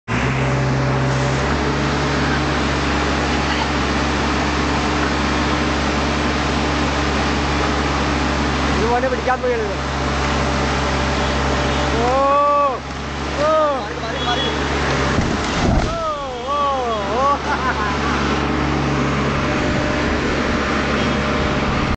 DismantleOfChruch.MP4
This is the video of a Crush of Church in Kochal, Koonammavu,Kerala,
See what happen at last,. ?